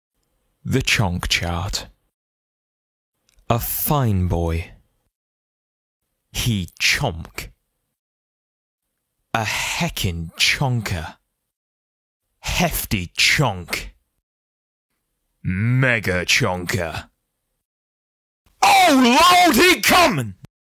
The Chonk Chart - Shiba Inu Edition
The Chonk Chart for Shiba Inu Dogs.

Featuring the following Shibas:
- Beau (of @Eeveebork )
- Kylo (of @KyloTheDoge )
- Eevee (of @Eeveebork )
- Meap (of @meap__thenotsogoodboi on Instagram)
- Heart (of @shibalove)
- Moja (of @moja412744 on Instagram)

Facts about Eevee the Shiba Inu (eevee.bork):
Name: Eevee
Age: 7
Gender: ♀
Fur color: Red
Birthday: Feb 12th